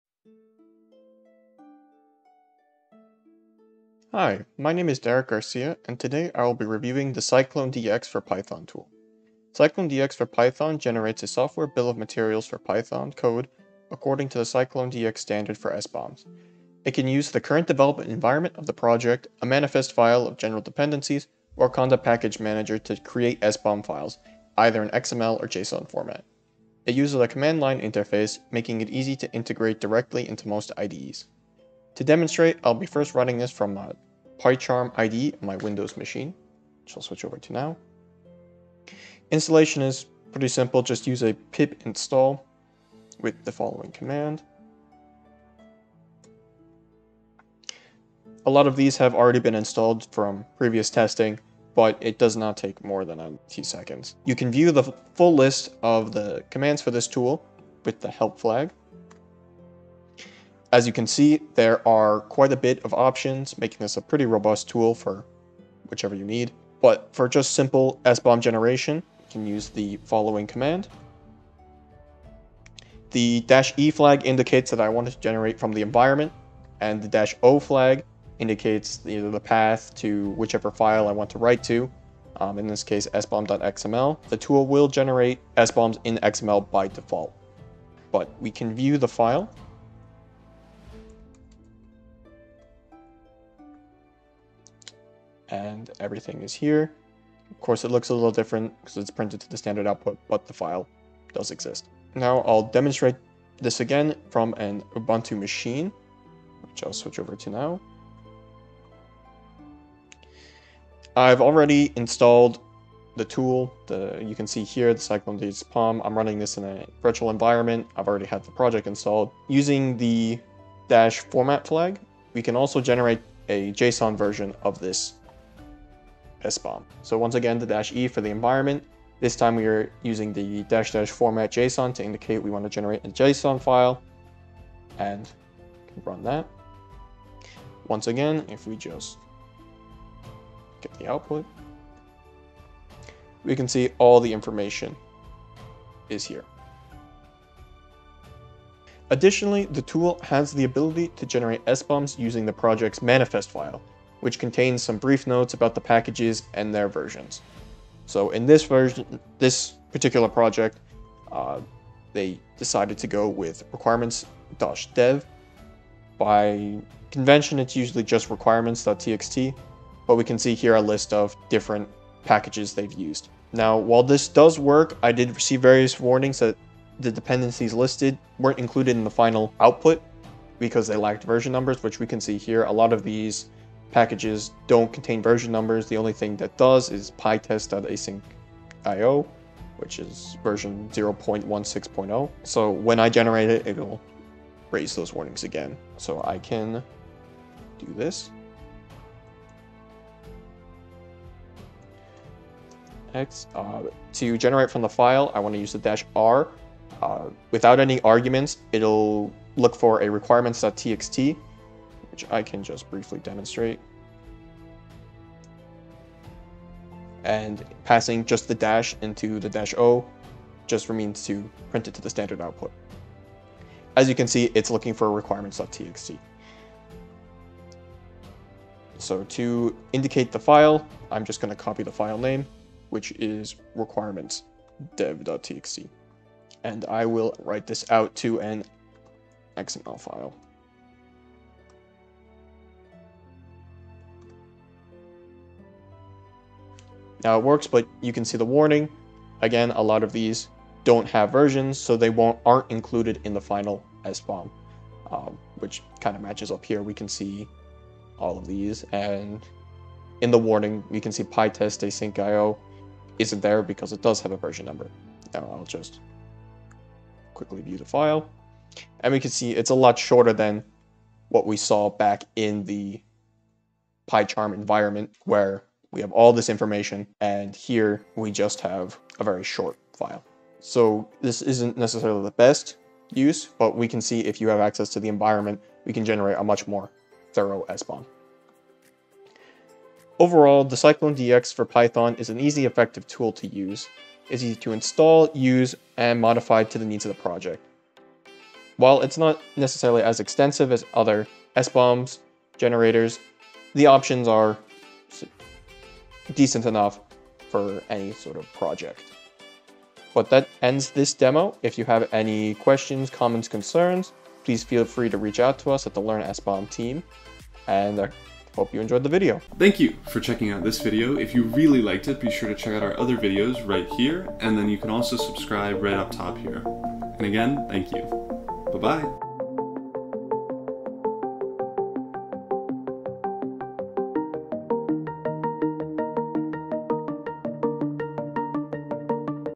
Tool Review: CycloneDX for Python
Summary
CycloneDX for Python is a Python Package that generates CycloneDX SBOMs for Python projects. It can run from numerous locations to better suit your development enviroment. In this video, we will demonstrate how to generate SBOMs using CycloneDX for Python.

Chapters
0:00 Intro
0:37 Installation
0:59 Command Overview
1:09 Generate an SBOM ( Windows )
1:48 Generate an SBOM ( Linux )
2:34 Generate an SBOM from a Manifest File
4:48 Final Thoughts
5:10 Thank You for Watching!

More Great SBOM Resources!